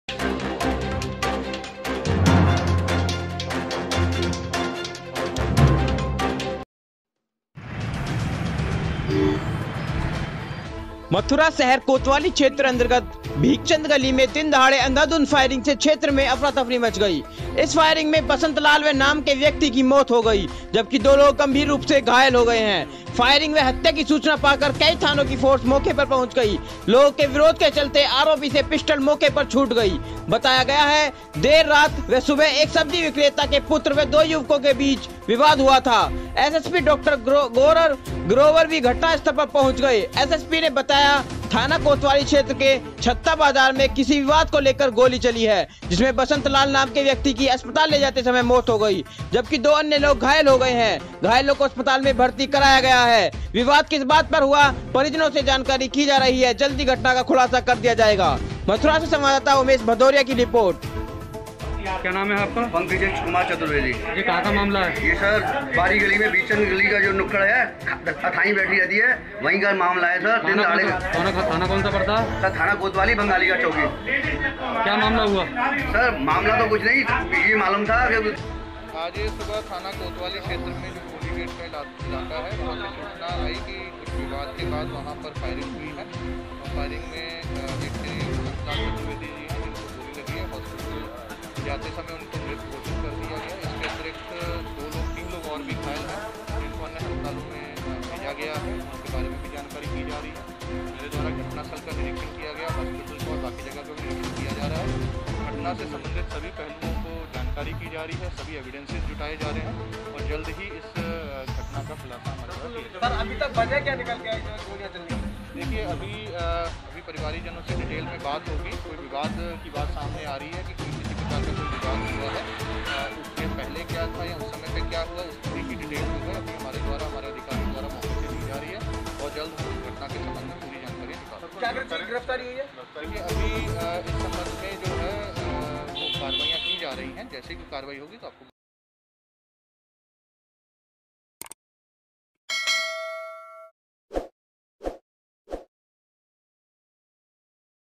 मथुरा शहर कोतवाली क्षेत्र अंतर्गत युवक पर दिनदहाड़े अंधाधुंध फायरिंग से दो युवकों की मौत
रिपोर्ट: उमेश भदोरिया मथुरा